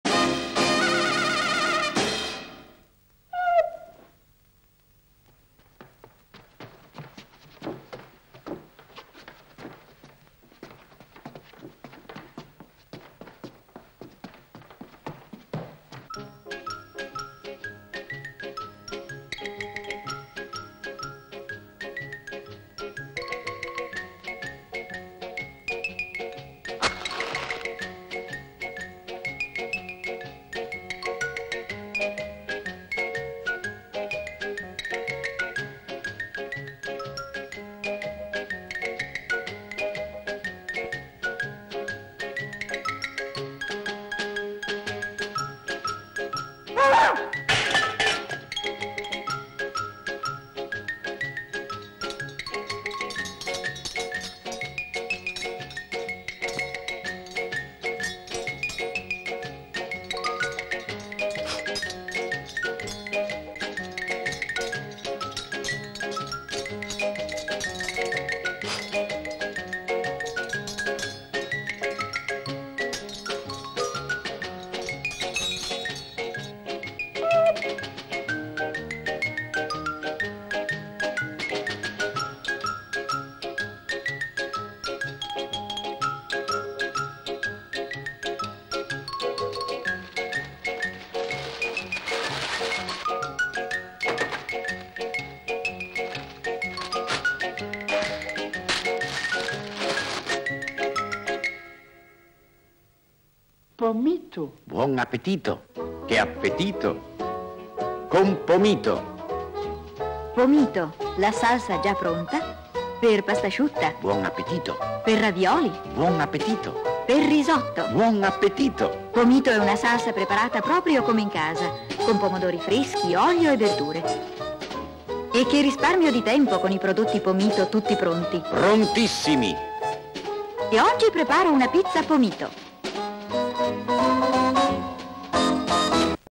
Salsa di pomodoro Pomito mac ronay show La candela
Spot Salsa di pomodoro Pomito mac ronay show La candela, casa di produzione PCT, anno prima messa in onda 1966